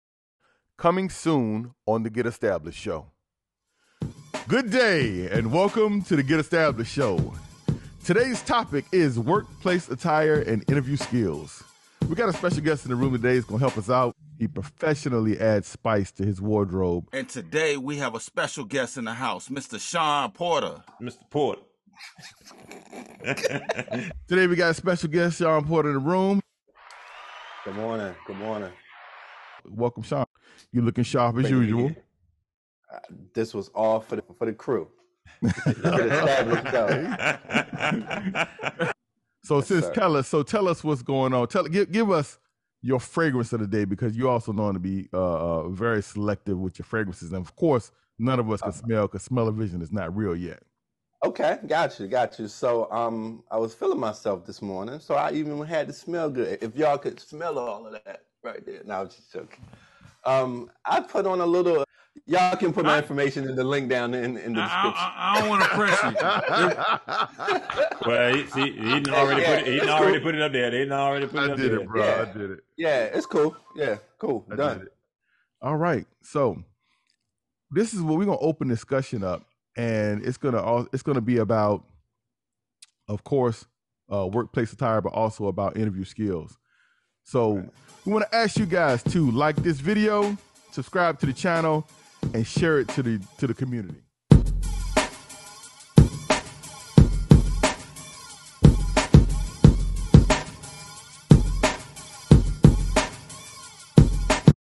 The Get Establish’d Show – Talking With “Mr. Bowtie Tuesday!”
In this show we experience tips and skills from Mr. BowtieTuesday Shawn L. Porter (Check out his YouTube channel, Shawn L Porter Photography). He helps THEGESCREW highlight how to dress for an interview along with how to professionally add spice to your wardrobe that could improve your upward mobility at work.
Tune-in... We can’t wait to share this fun and educational Full Show coming soon.